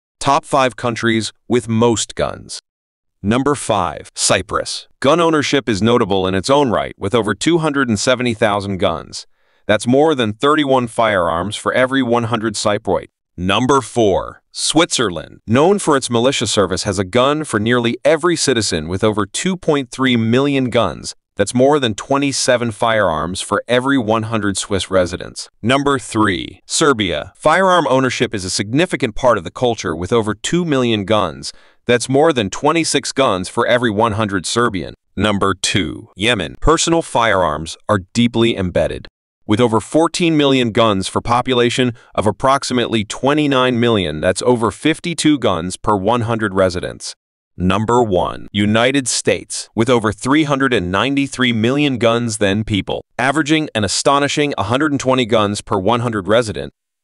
Top 5 Countries with Most Guns
In this video, we explore the top 5 countries with the highest number of guns per capita. From Cyprus to the United States, these nations have fascinating gun ownership statistics that shed light on their unique cultures and attitudes towards firearms